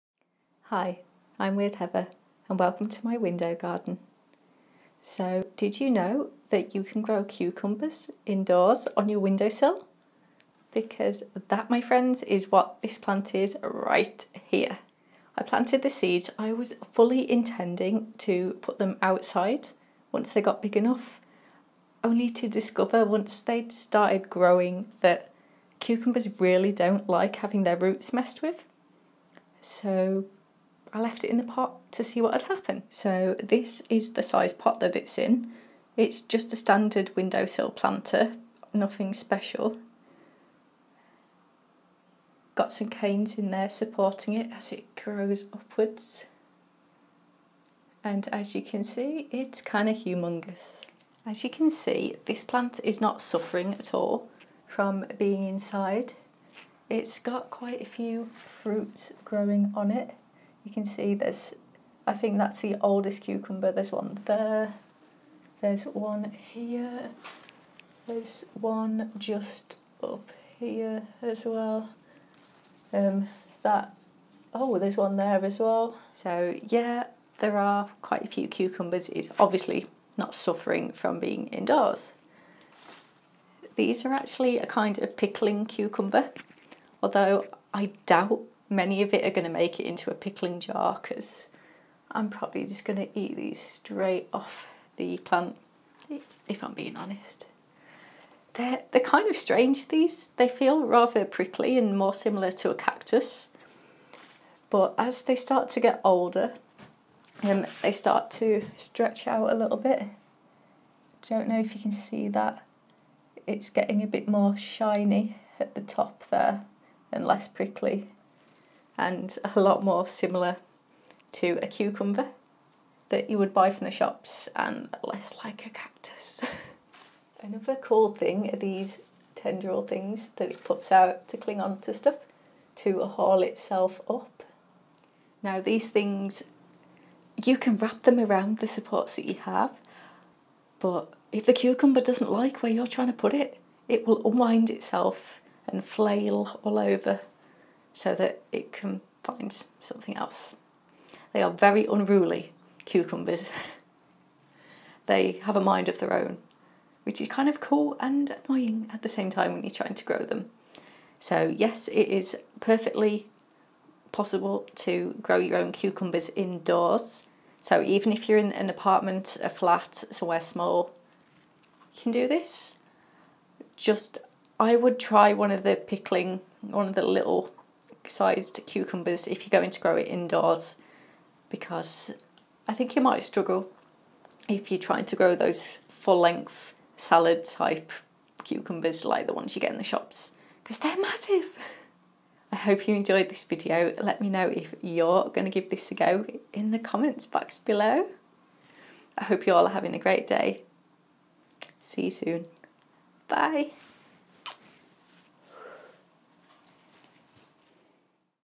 Growing INDOOR Cucumbers on your WINDOWSILLS!!!
Did you know you can grow indoor cucumbers on your windowsills? So if you live in a flat or apartment, cucumbers can be perfectly happy in a windowsill planter in one of your sunny windows. I’d recommend growing the smaller, pickling varieties of cucumber if you’re growing on your windowsill, like I am in this video. The variety I’m growing is called Vert Petit De Paris.